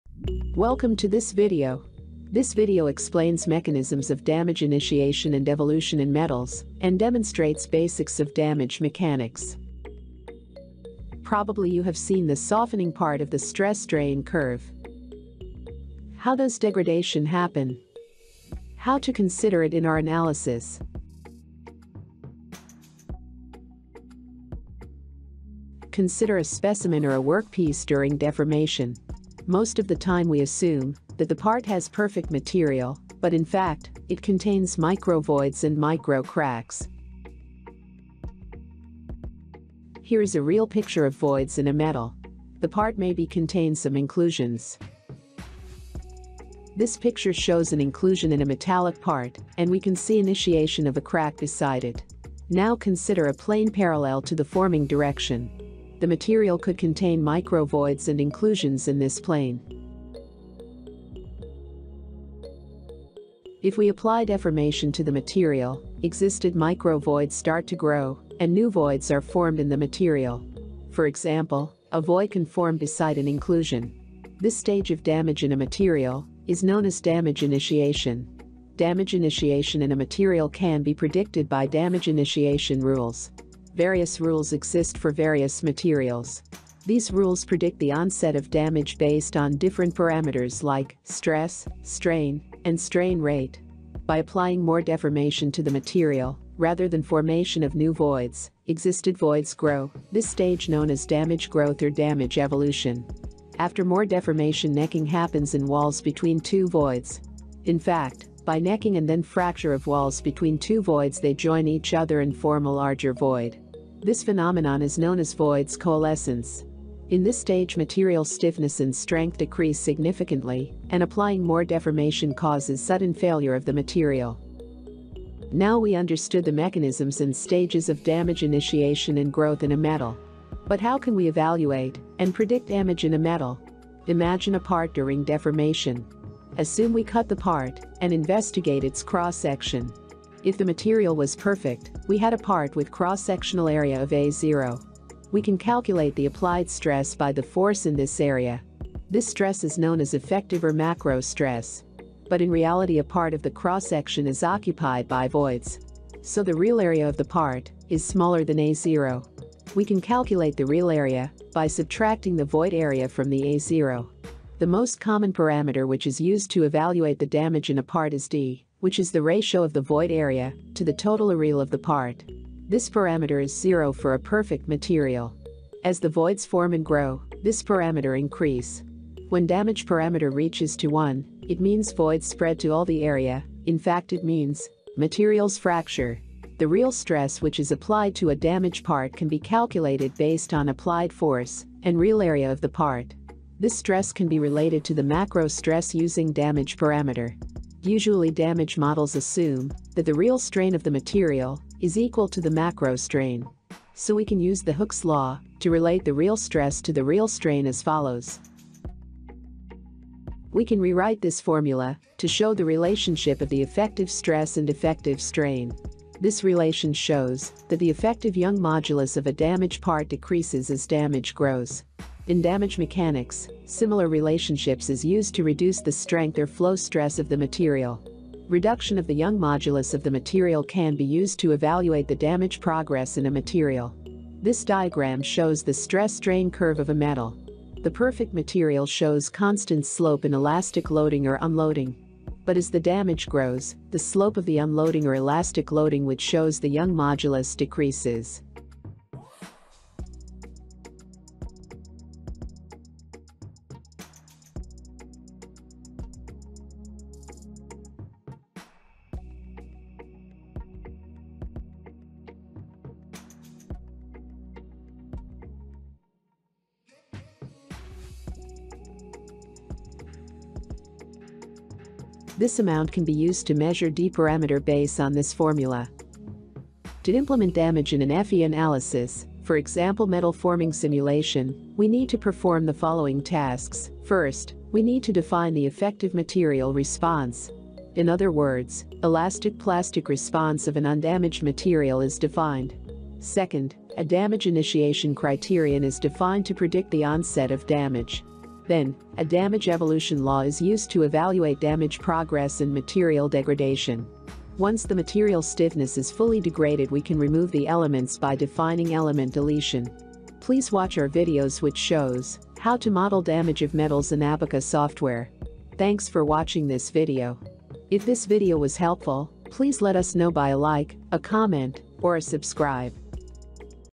Understanding continuum damage mechanics
This video explains mechanisms of damage initiation and evolution in metals and demonstrates basics of damage mechanics. In this video you can find out:
What are damage stages in a metal?
What is damage initiation?
What is damage evolution?
What is material degradation?
What is void initiation?
What is void growth?
What is void coalescence?
How to evaluate damage?
What is damage parameter?
How to measure damage parameter?